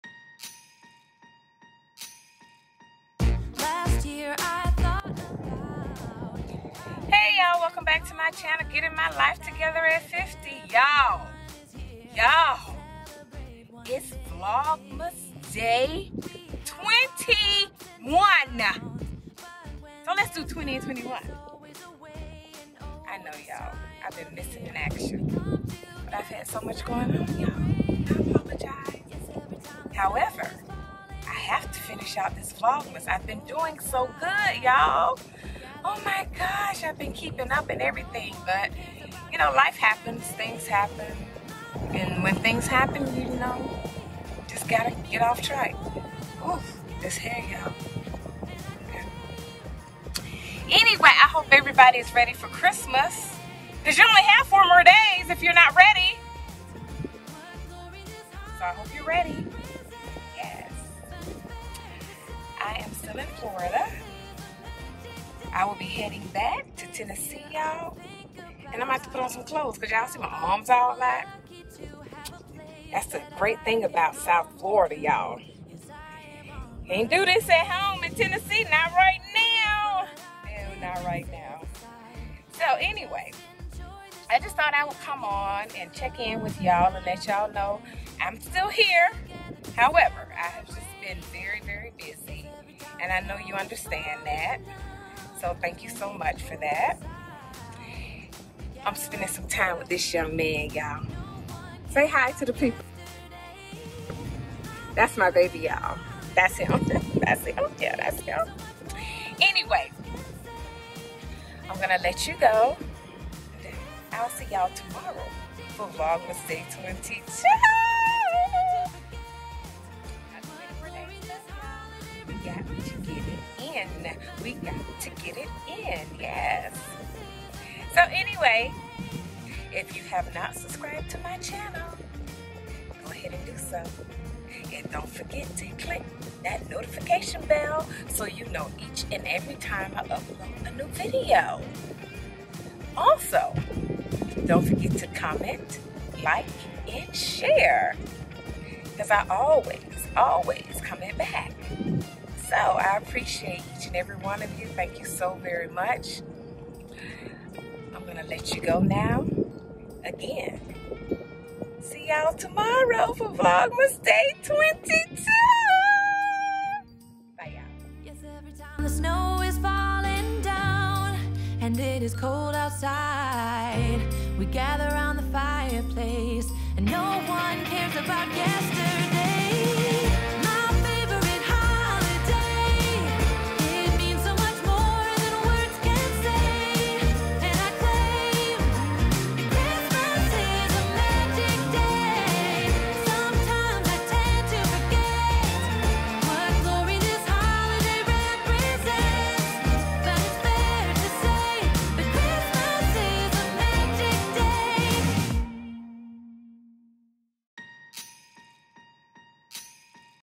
Vlogmas Day 20&21: Still in South Florida
Vlogmas Day 20&21: Still in South Florida, Spending time with my Son, Saying bye to South Florida until next time, Life Happens, Determined to finish 25 days of Vlogmas